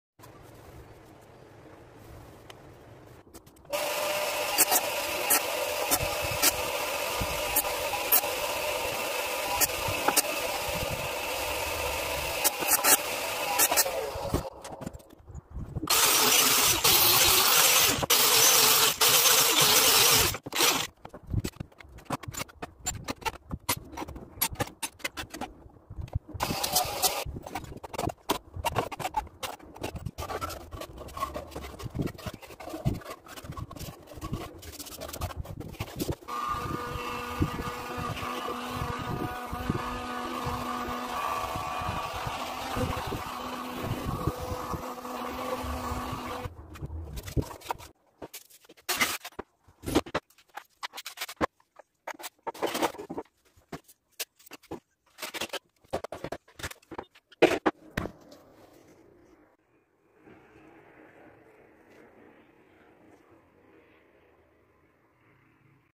Ahsap Love Raf Yapımı Hediyelik Eşya Raf
Artık Malzemelerden Dekoratif Ve Kullanışlı Raf Imalatı , Askılik